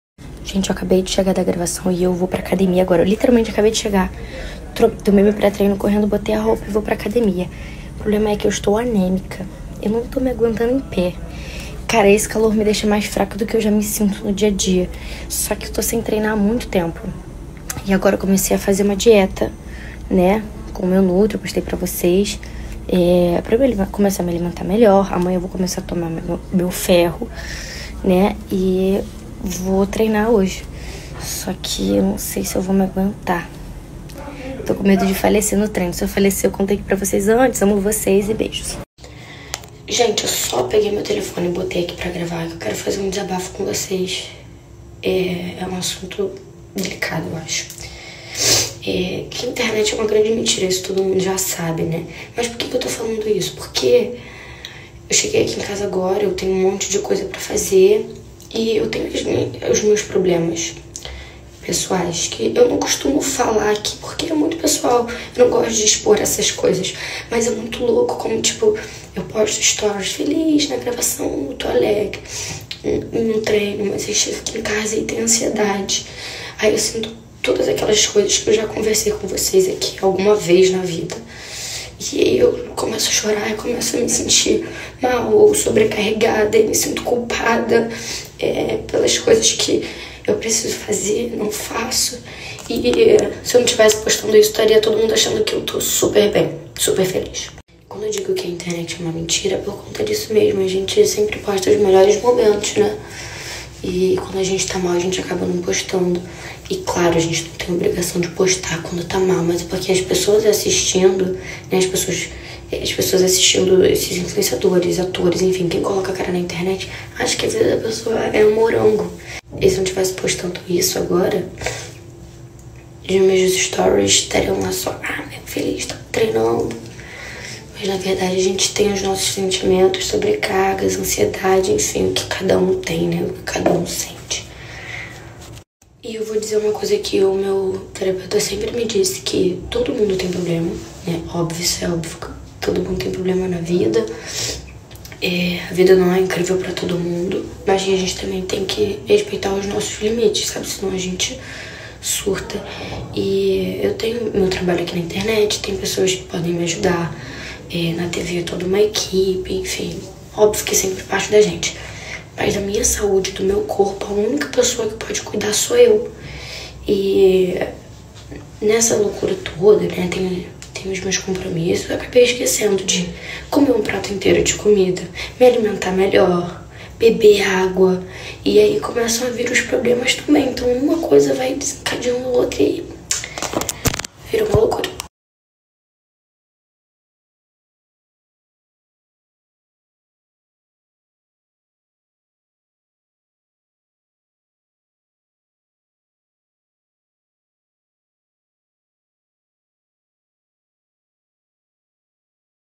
Mel Maia desabafa sobre ansiedade
A atriz Mel Maia, de 18 anos, usou seus Stories do Instagram, na noite de segunda-feira (13), para fazer um desabafo com seus seguidores sobre a ansiedade.

Vídeo: reprodução Instgram @melissamelmaia